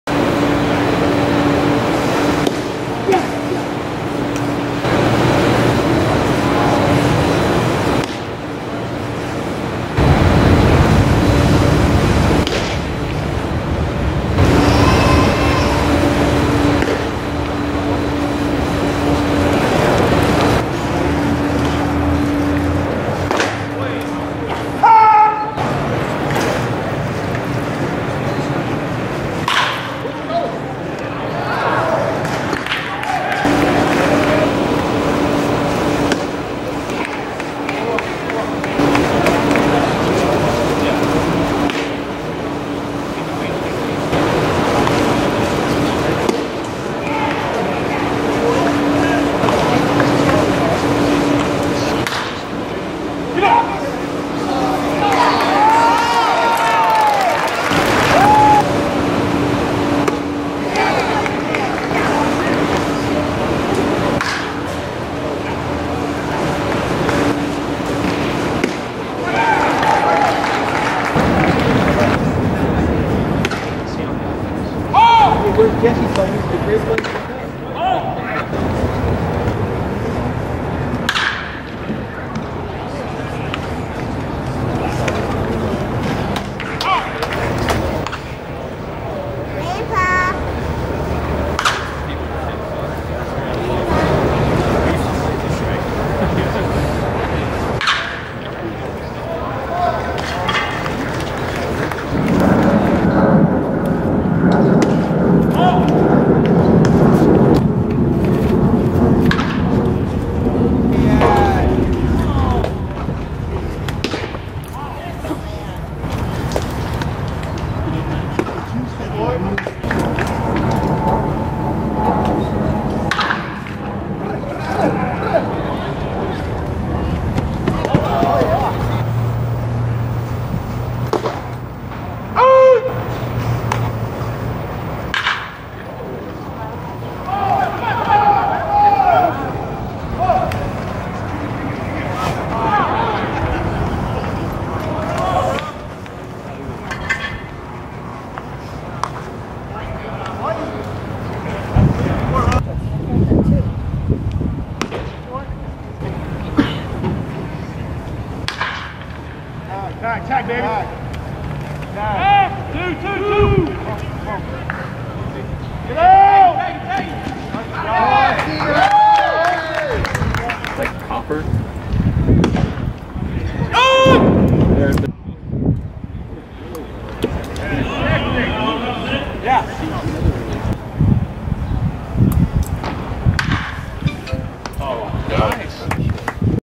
Mauricio Ramos, Kansas City Royals INF Prospect (2016 Arizona Fall League)
Several of Ramos' (6'1" 185 lbs, Cartagena, Colombia) plate appearances with the Surprise Saguaros in the 2016 Arizona Fall League.

Billy Parker Field at Surprise Stadium, Surprise, AZ
November 7, 2016
Sloan Park, Mesa, AZ
November 8, 2016
Camelback Ranch, Glendale, AZ